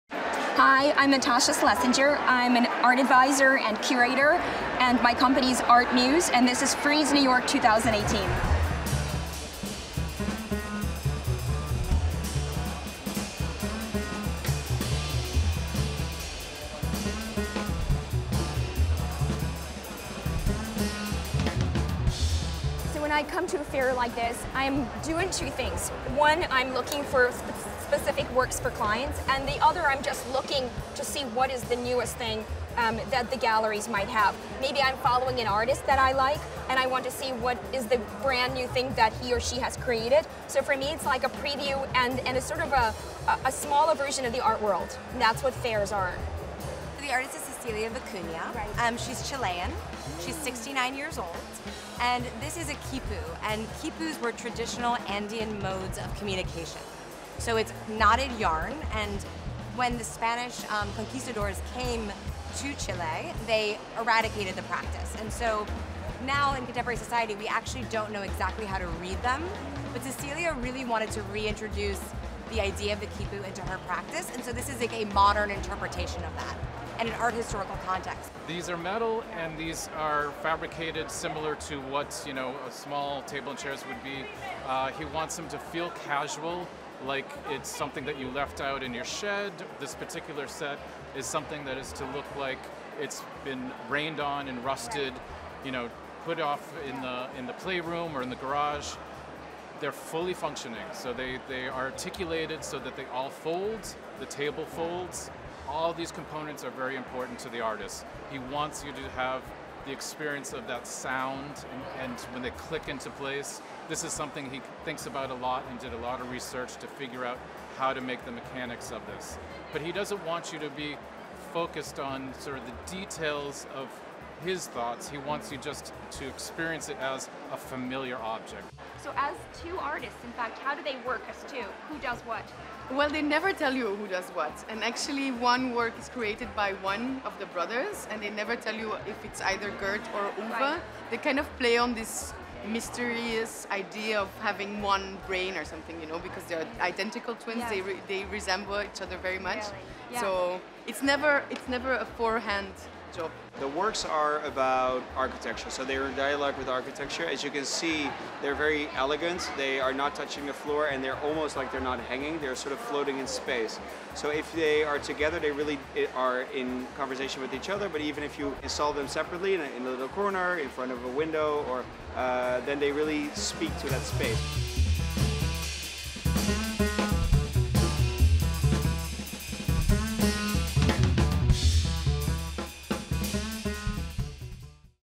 Frieze New York 2018
We took a tour of the Frieze New York 2018 art fair on Randall's Island with art advisor and curator Natasha Schlesinger, founder of ArtMuse.